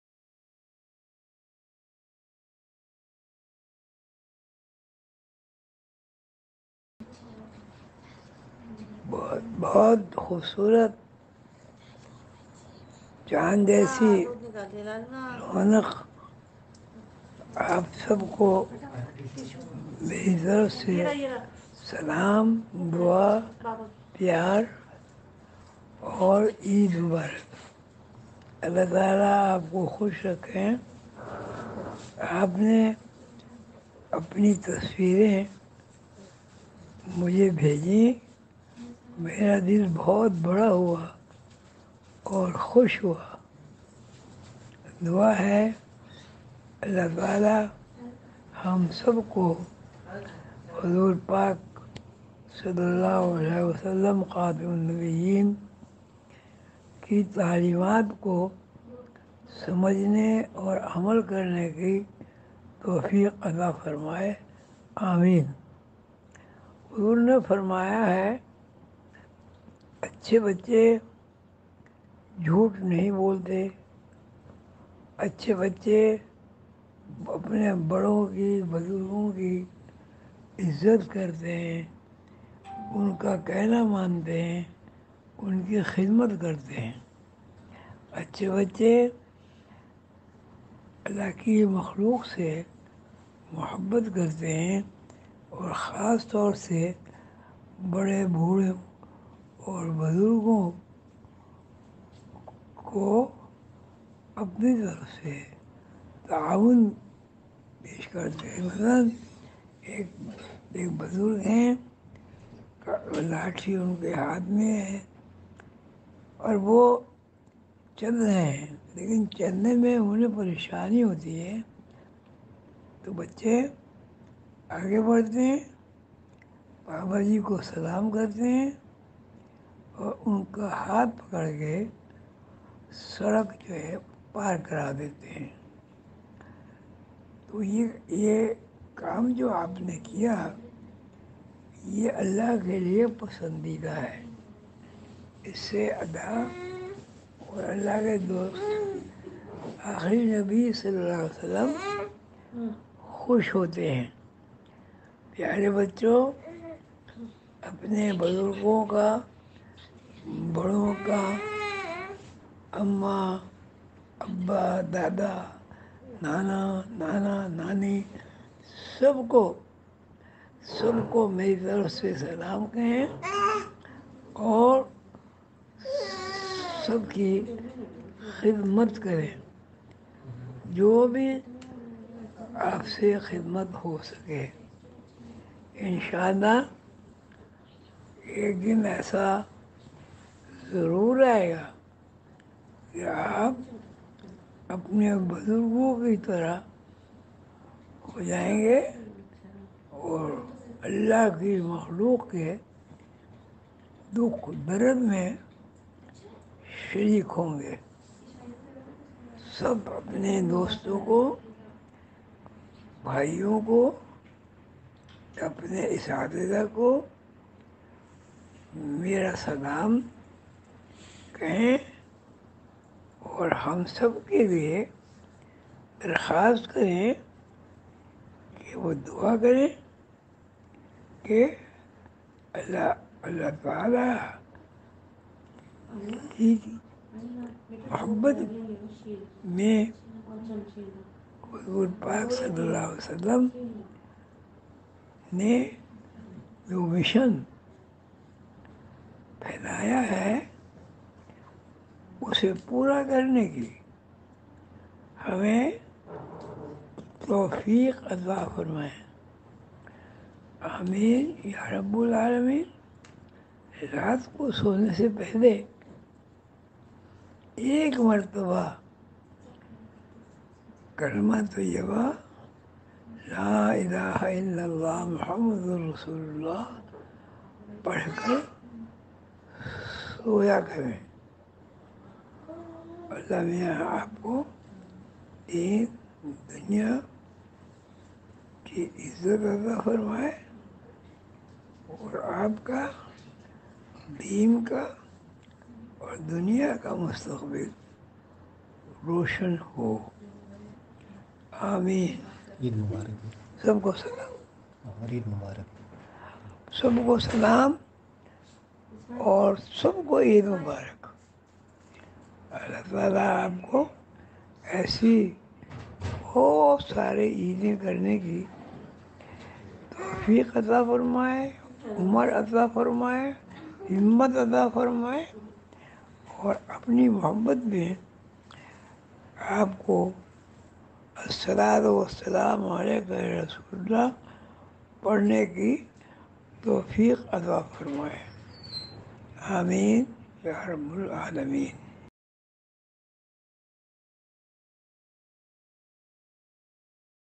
Khwaja Shamsuddin Azeemi Eid Message for Children
محترم خواجہ شمس الدین عظیمی کا بچوں کے نام عید پر پیغام
(یہ پیغام شمالی امریکہ میں مقیم عظیمی بچوں کی طرف سے عید پر مبارکباد کے پیغام کے جواب میں ریکارڈ کیا گیا)
Asslam u Alaikum ! lots of duas, love, and Eid Mubarak to all beautiful children just like the moon. May Allah keep you all happy. You have sent your pictures (videos) to me which filled my heart with great happiness. I pray that may Allah make all of us understand and act upon the teachings of Prophet Muhammad (PBUH). Prophet Muhammad said that the good kids always pay respect to their elders. They also serve and obey their elders and never tell lies. Good Children also love Allah’s creations. Especially, they offer their cooperation to help the elderly senior citizens. For instance, if an old person, holding a wooden stick in his hand, has difficulty walking and needs help, kind kids would step forward, say salam to him and help him in crossing the road. Allah (SWT) likes such acts of kindness. Allah and Friends of Allah Prophet Muhammed (PBUH) become happy with such deeds.
Dear children!  say my Salam to your elderly seniors, grandparents, and parents and serve them all. Whatever strength you have, use it to serve others. InshaAllah, in the future, you will be like your elders and be sympathetic to share the pain of others. Again Say my Salam to your friends, brothers, sisters, and teachers. Also, ask each one of them to make duas for all of us to achieve Allah's love and the strength to accomplish the mission of our prophet Muhammad (PBUH) Ameen ya Rabul Ameen.
Dear Children!  recite Kalma tayybaa before you sleep. May Allah grant you the
glory of this deen and dunia and the future of your deen and dunia will also be brightened.  Ameen !
 Salam and Eid Mubarak to everyone. May Allah give you opportunities to celebrate many more Eids and bestow you long life.  May Allah also give you the strength to recite Darood with love. Ameen Ya Rabb ul Almeen .